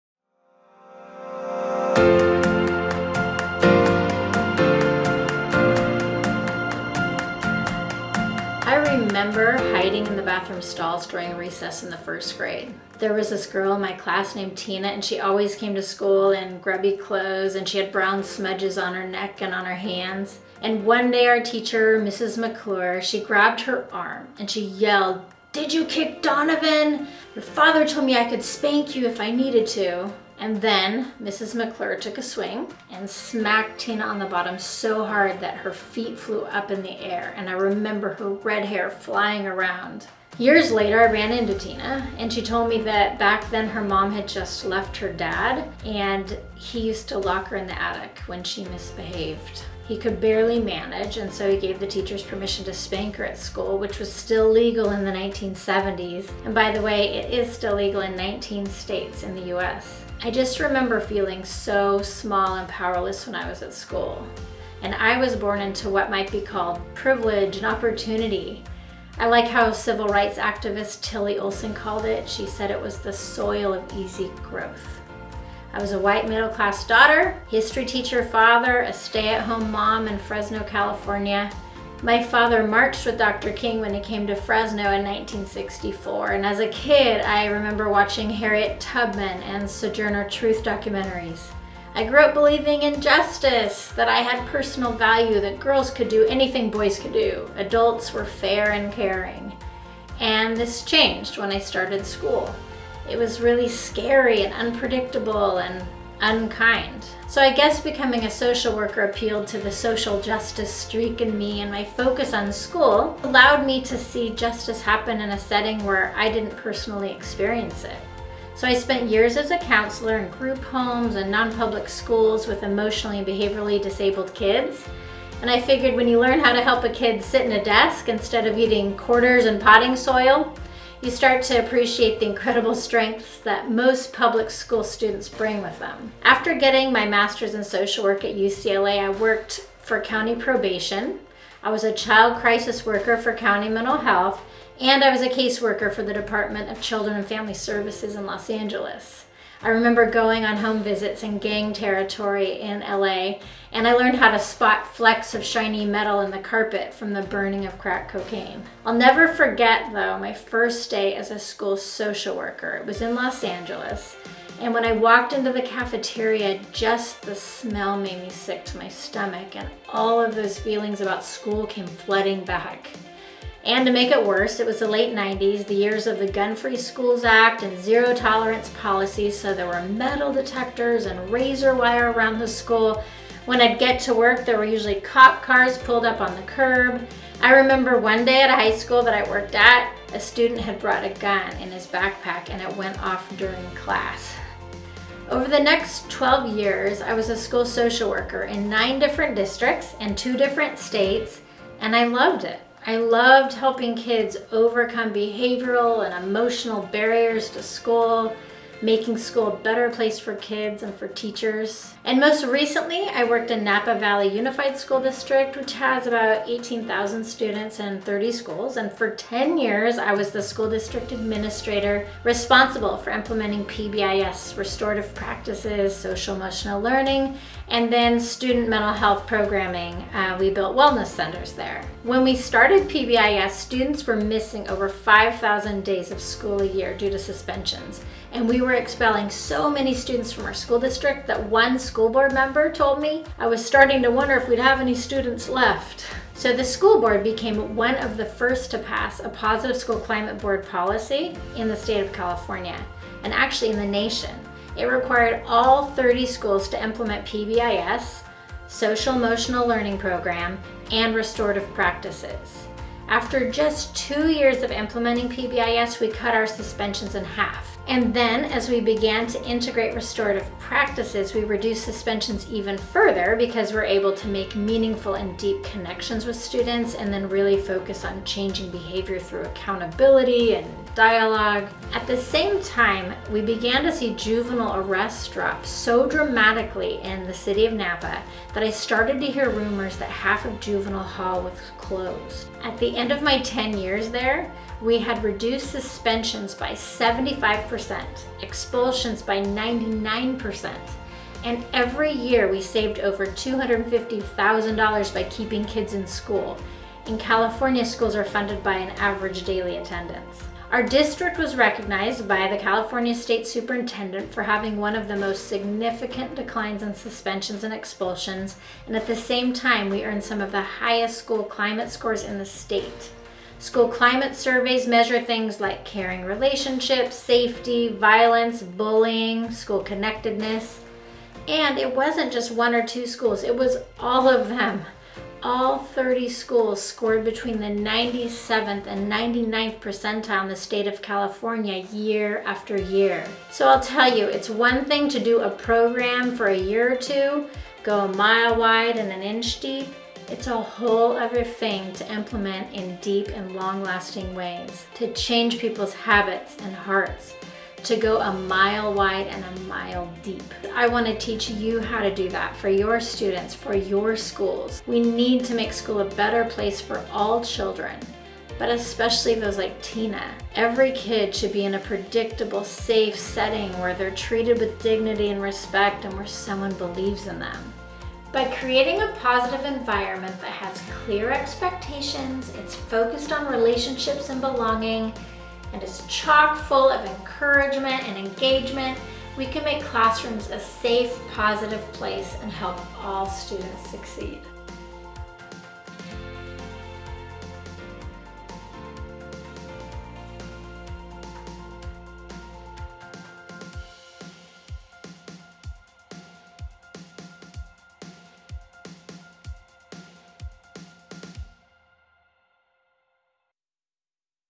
Meet Laura Mooiman
Hi, I wanted to introduce myself and give you a little background on my journey that led me here: helping schools create safe, positive, and restorative environments for their teachers and students.